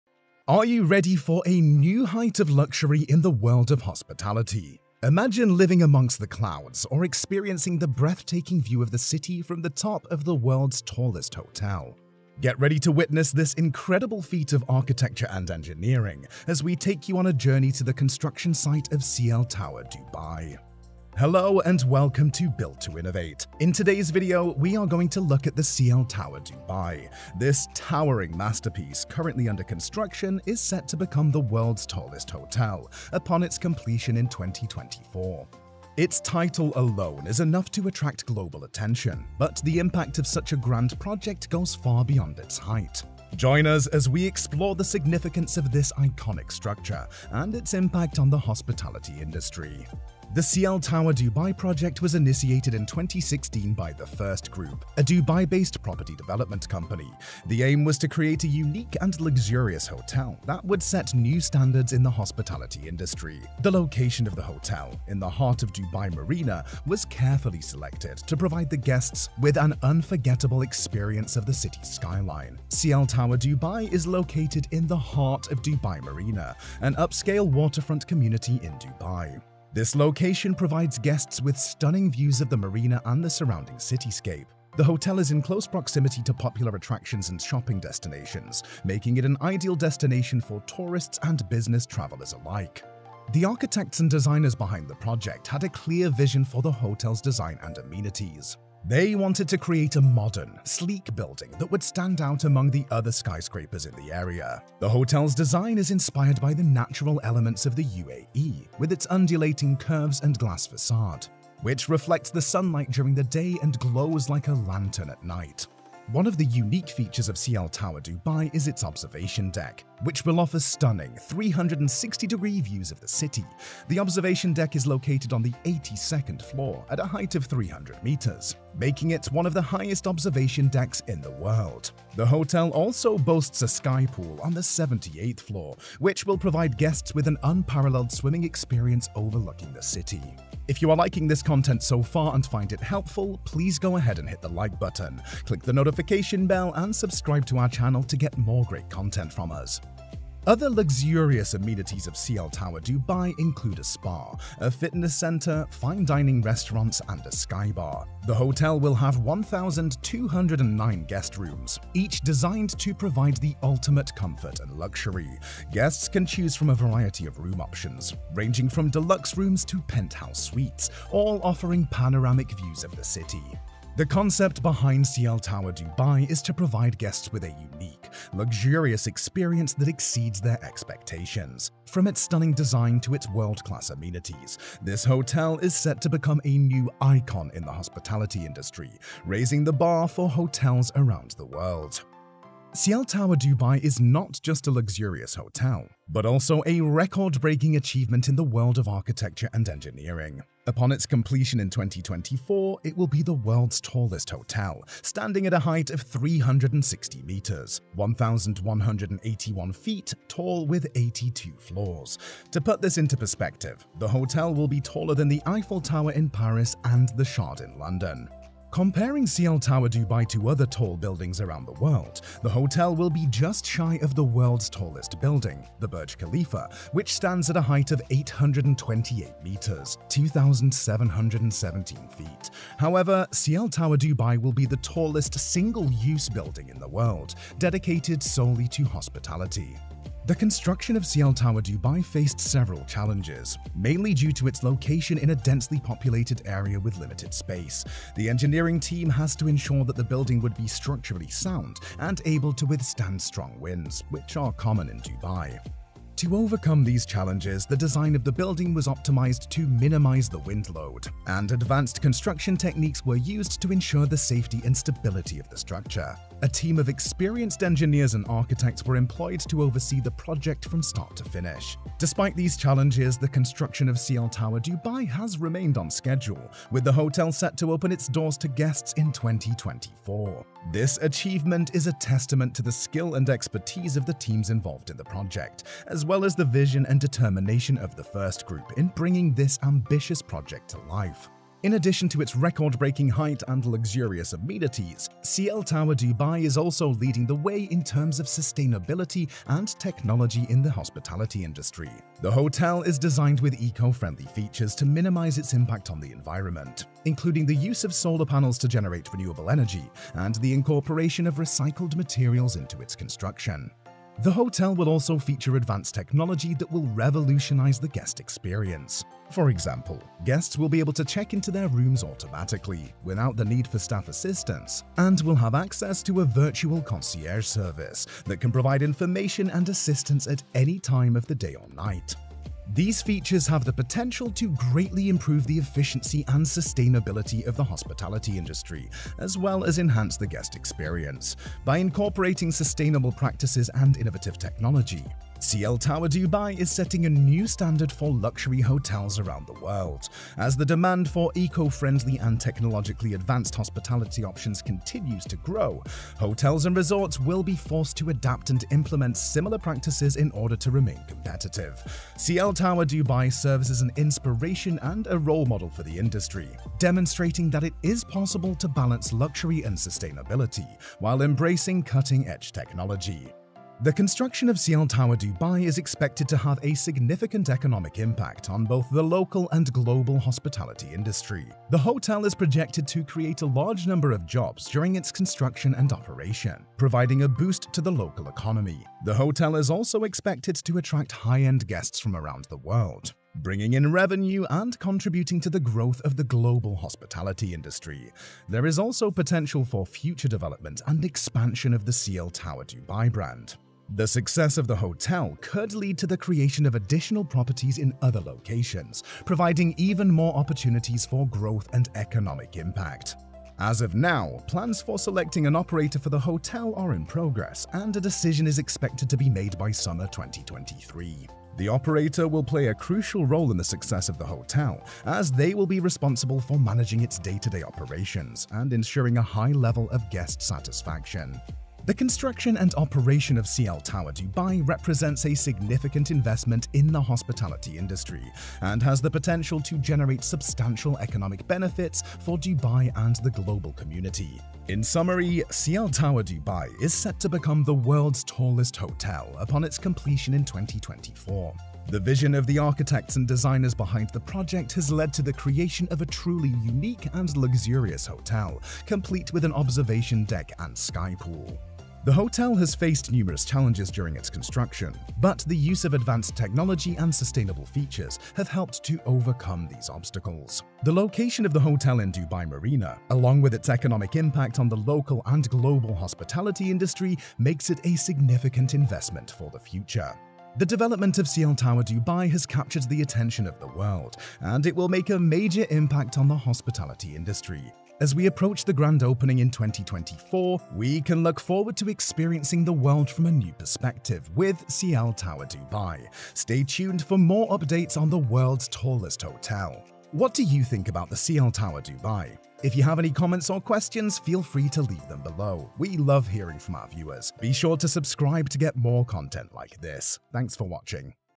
World's TALLEST HOTEL Under Construction to OPEN in 2024!
Be the first to know about the world's tallest hotel under construction! Imagine living amongst the clouds or experiencing the breathtaking view of the city from the top of the world's tallest hotel. Get ready to witness this incredible feat of architecture and engineering, as we take you on a journey to the construction site of Ciel Tower in Dubai.

In today’s video we are going to look at the Cie Tower set to open in Dubai in 2024. Which is s set to become the world's tallest hotel upon its completion in 2024. The Ciel Tower Dubai project was initiated in 2016 with the aim to create a unique and luxurious hotel that would set new standards in the hospitality industry.

0:00 The World's Tallest Hotel The Ciel Tower
0:55 What is the Ciel Tower Hotel?
2:00 The Design of the Ciel Tower
3:20 A Record Breaking Hotel Tower
5:10 An Advance Hotel Unlike Never Before
6:30 A New Luxury Hotel in Dubai
7:30 The World's Tallest Hotel for 2024